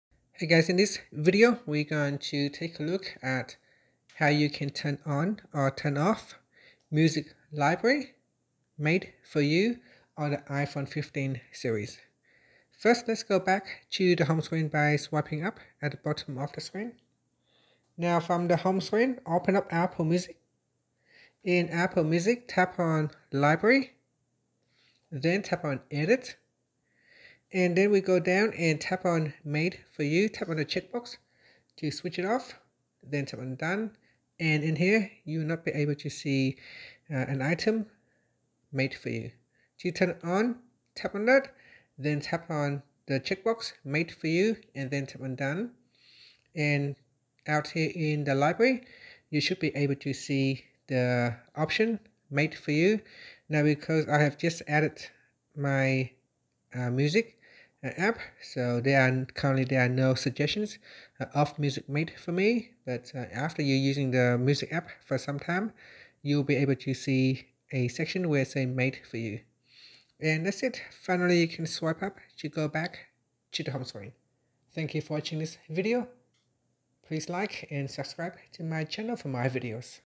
iPhone 15/15 Pro Max: How to Turn On/Off Music Library Made For You
Welcome to our guide on how to effortlessly manage your Music Library on the latest iPhone 15 series running iOS 17! In this tutorial, we'll show you how to turn on and off the "Made For You" feature, ensuring your music experience is tailored just the way you like it. Whether you're using the iPhone 15, iPhone 15 Pro, iPhone 15 Pro Max, or the iPhone 15 Plus, this step-by-step walkthrough has got you covered.